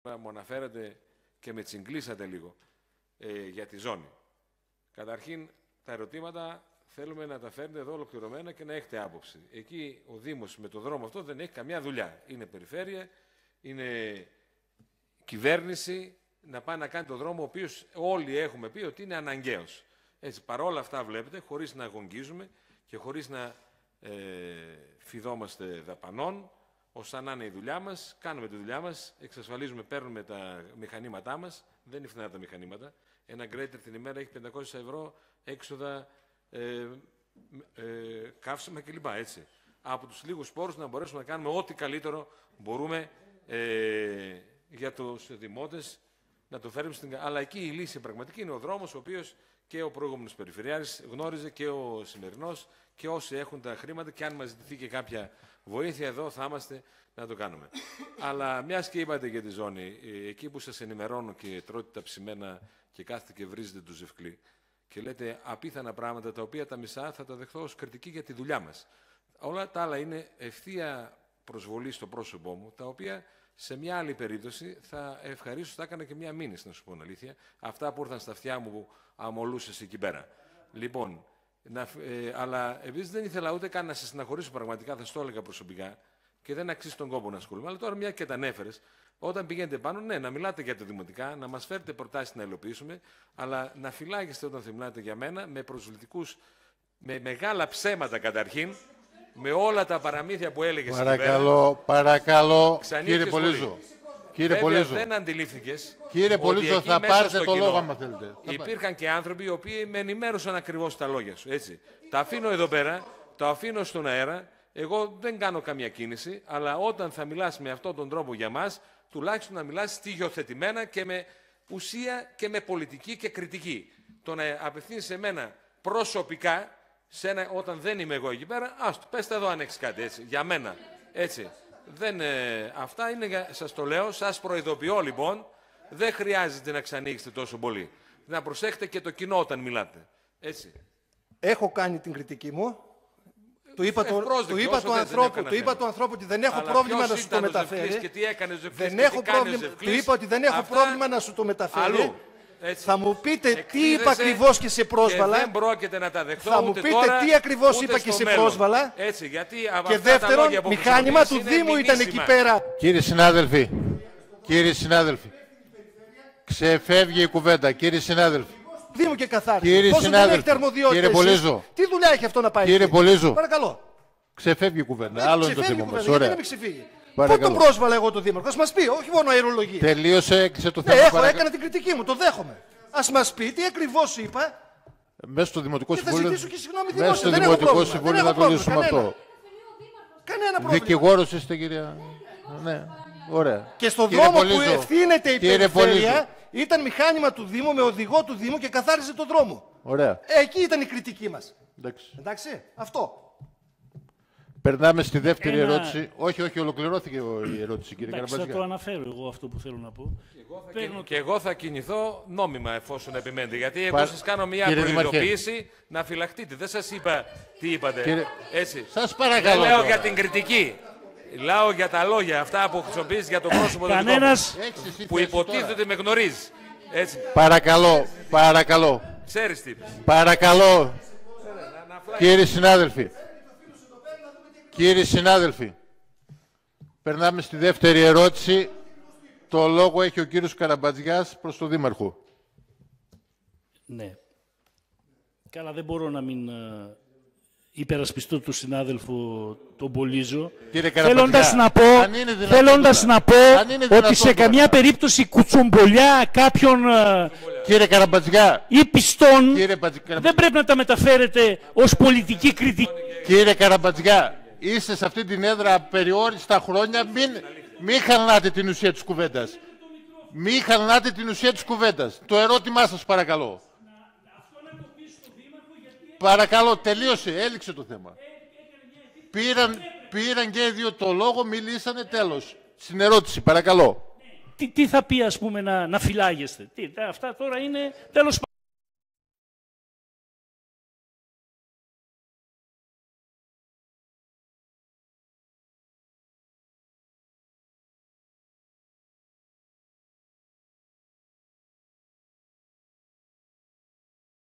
kozan.gr: Δήμαρχος Βοΐου Χ. Ζευκλής σε δημοτικό σύμβουλο της Λαϊκής Συσπείρωσης
Μια ερώτηση του δημοτικού Συμβούλου της Λαϊκής Συσπείρωσης Χ. Πολύζου σε σχέση με κάποιο δρόμο στη Ζώνη Βοΐου κι αν υπάρχει εμπλοκή του Δήμου Βοΐου στο συγκεκριμένο έργο στάθηκε αφορμή για να "ξεσπάσει" ο Δήμαρχος  X. Ζευκλής κατά του πρώτου, στην ειδική συνεδρίαση λογοδοσίας, το βράδυ της Τρίτης 29/10, λέγοντάς του ότι σε ιδιωτική συζήτηση που είχε με κάποιους άφησε υπονοούμενα κι άλλες κατηγορίες για τον Δήμαρχο . Δείτε τι συνέβη στο χαρακτηριστικό απόσπασμα που σας παρουσιάζει το kozan.gr